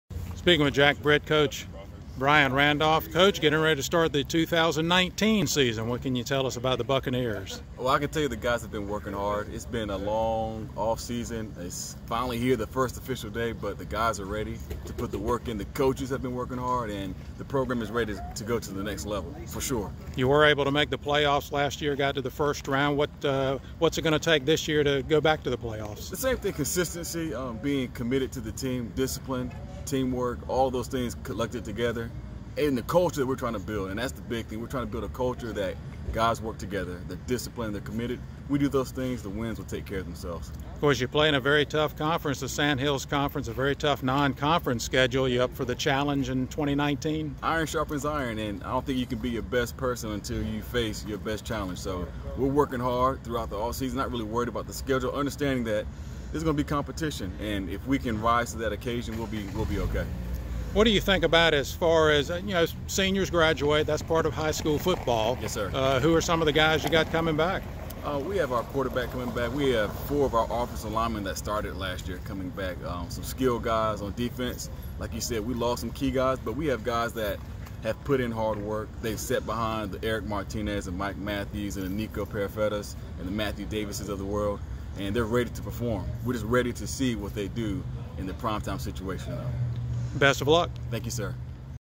Jack Britt's first day of practice 2019
Jack Britt football coach Brian Randolph talks about the upcoming season on Thursday, Aug. 1, 2019.